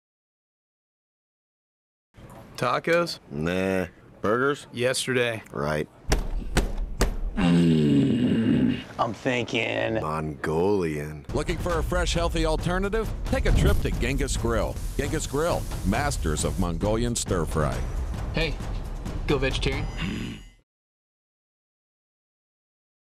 Will Koberg in "Lunch at Genghis Grill"
Will Koberg in Genghis Grill commercial.